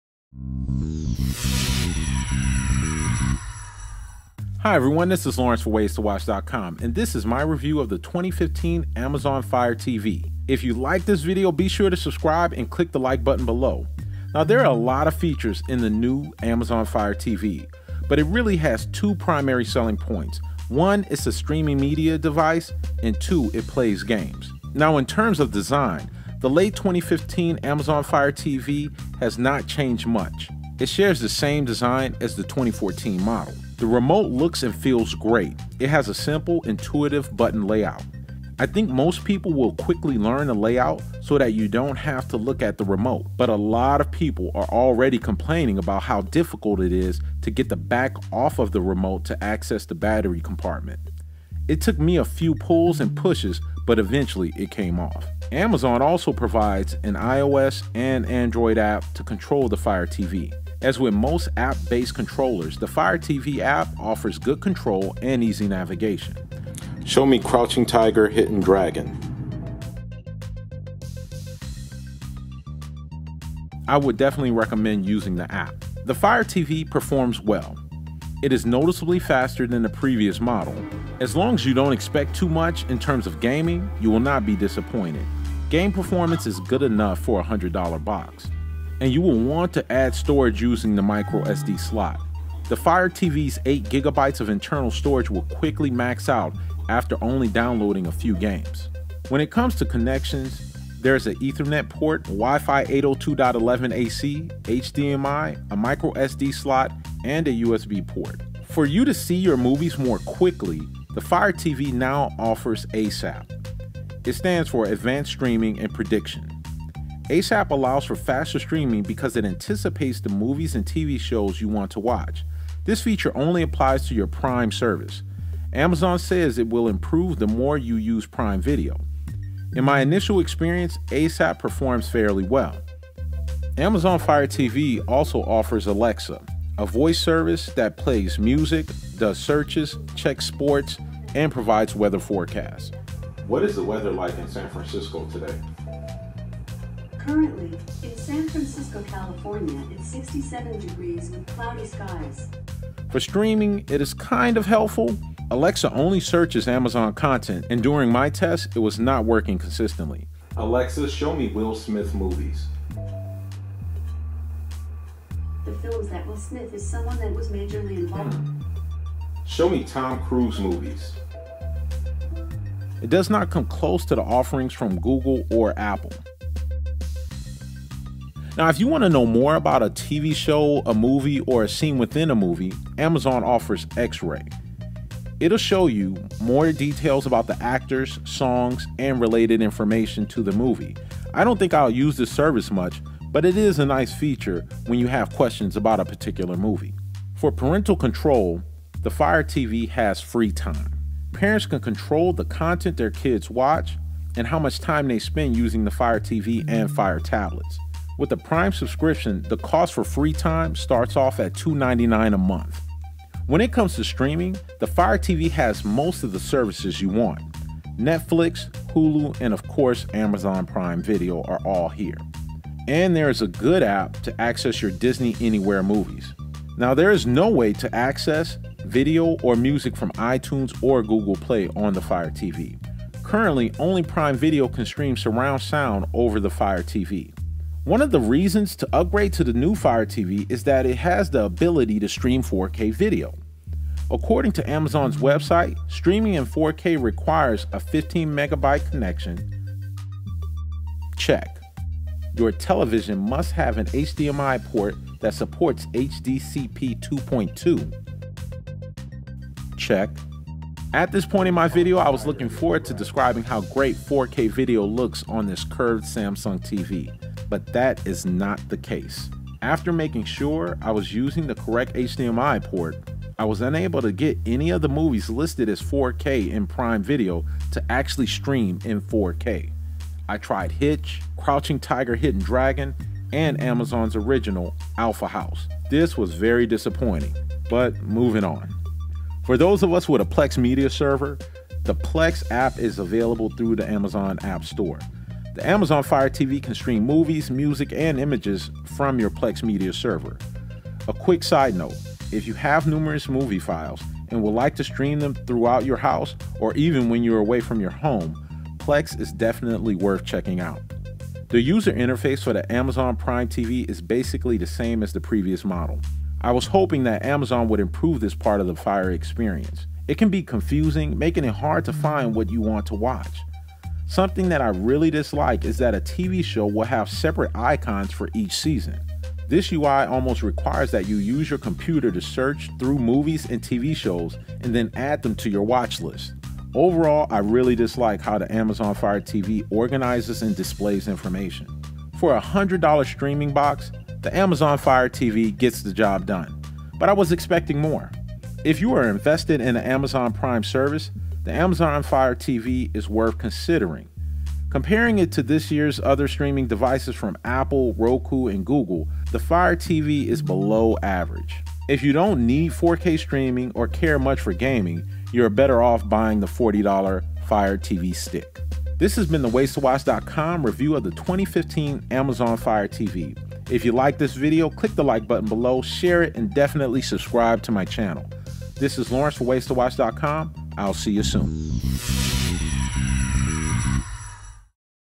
New Amazon Fire TV (2015) Review!!
This is the WaysToWatch.com review of the 2015 Amazon Fire TV. Does it live up to the hype? How does it rank amongst the new streaming devices from Google and Apple? It features the same design from the previous model. There is not much more than a square box. But who cares about the exterior? It is about how it feels and performs. The remote is well made, but there are issues with the back of the remote. It is hard to remove it. After working through the back of the remote, the remote actually feels really good in my hand. It works well and is really responsive. The iOS Fire TV App is helpful and easy to use. It performs well for streaming, but is limited when it comes to gaming. Many people are searching Amazon Fire TV review 4k, because that is one of the top selling points of this device. 4k did not work too well for me. Watch this review of the 4k potential of the Amazon Fire TV.   Amazon's ASAP feature aims to eliminate buffering time by assuming what you will watch. Amazon's Alexa voice service can play music, do searches, check sports scores, and provides weather forecast. Another feature from Amazon for the Fire TV is X-Ray. X-ray gives you information about a movie or TV show, like which actors are in a particular scene. Parent can use FreeTime as their parental control tool. All of the main streaming services are available on the Fire TV. Netflix, Hulu and Amazon Prime work on Fire TV. If you have a Plex Media Server, good news, the Fire TV can stream movies from the server. The user interface on the Fire TV can be confusing, overall it is a decent streaming box.